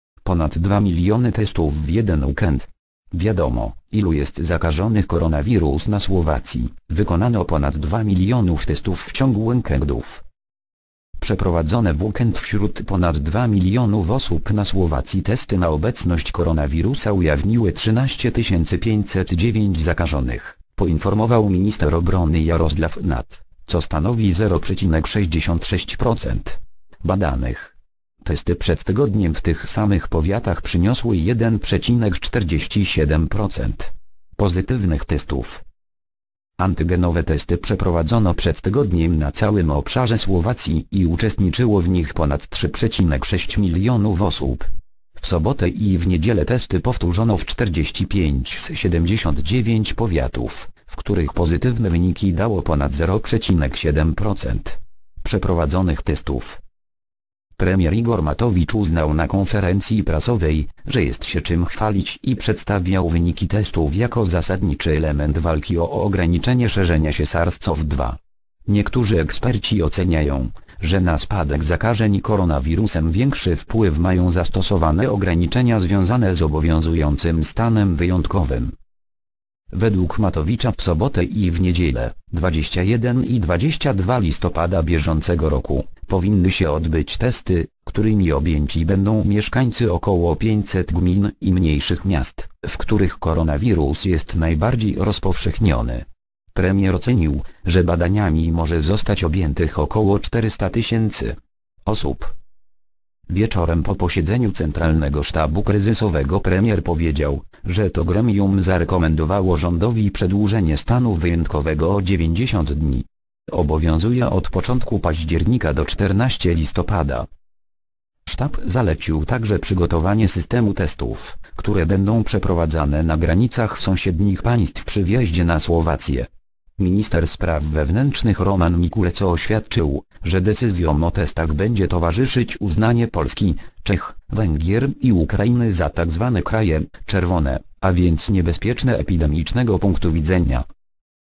Ponad dwa miliony testów w jeden weekend. Wiadomo, ilu jest zakażonych.
Koronawirus na Słowacji: Wykonano ponad 2 mln testów w ciągu weekendu.

  (fot. AFP)